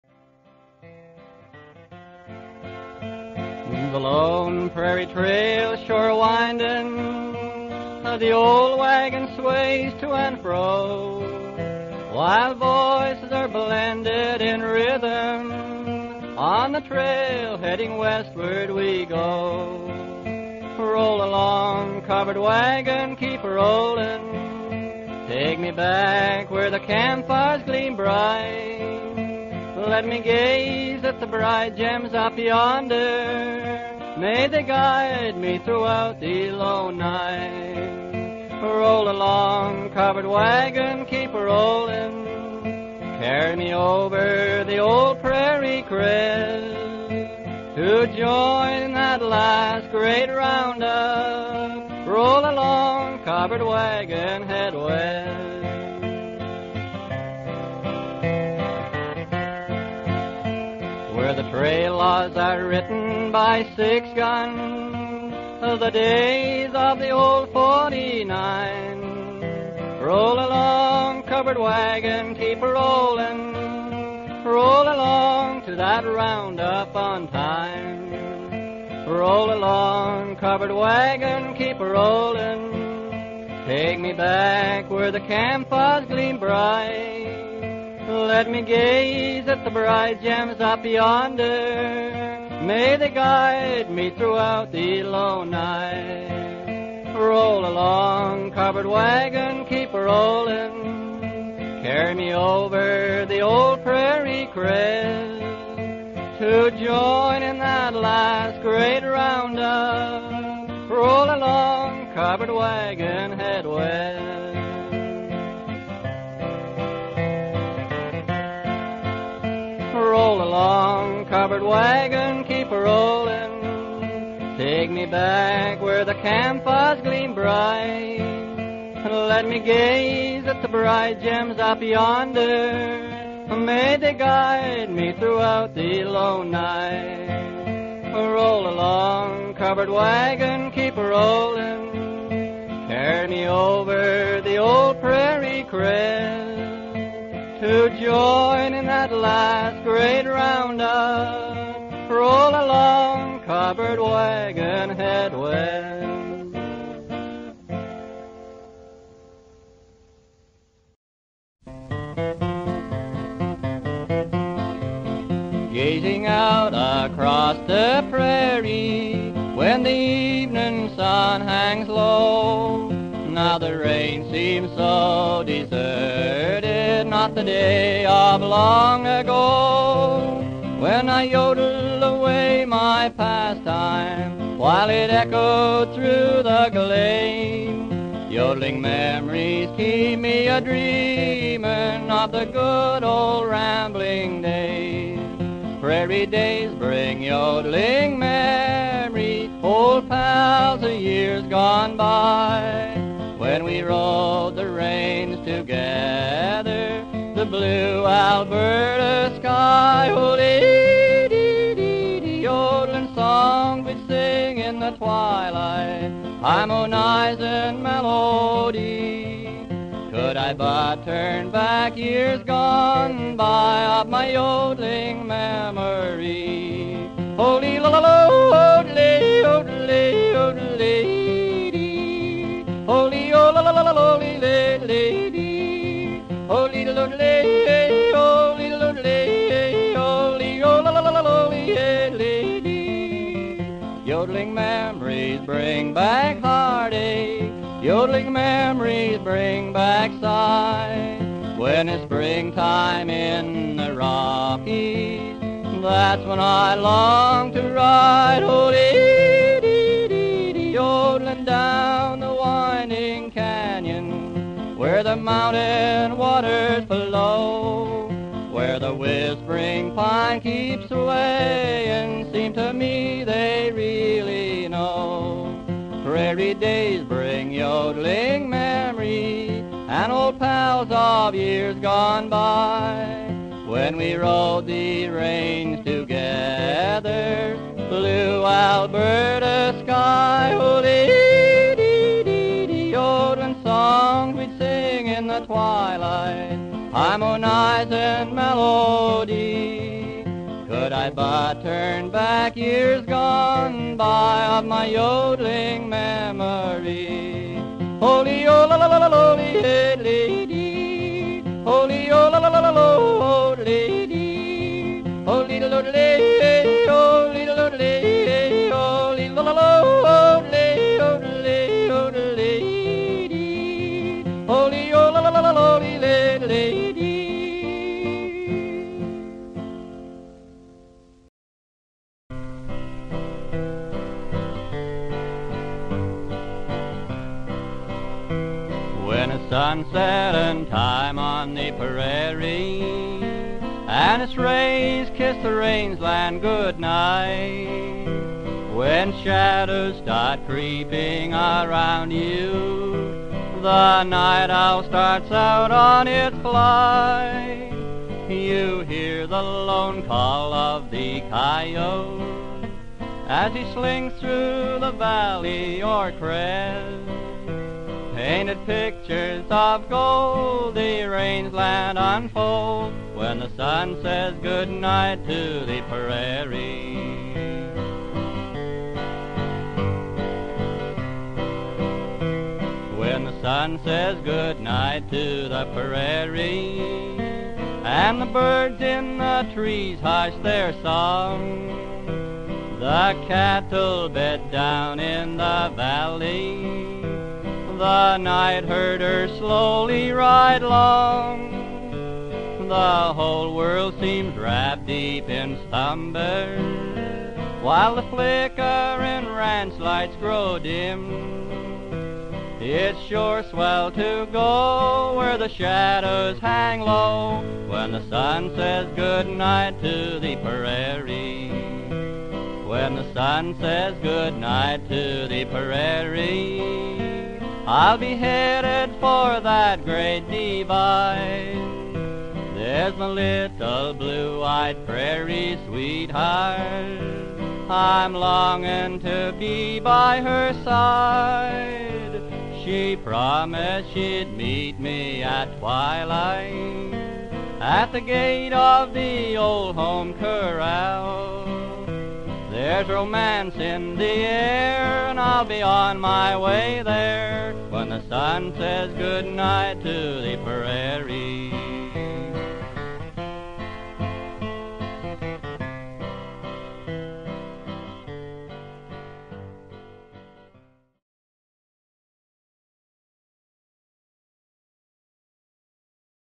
"West" - by Mark Schacter (watch in 1080p HD)
Take an 8-minute tour through the American and Canadian west, as seen by Canadian photographer Mark Schacter, and accompanied by the music of Wilf Carter ("Montana Slim"). Set your video player to 1080p HD.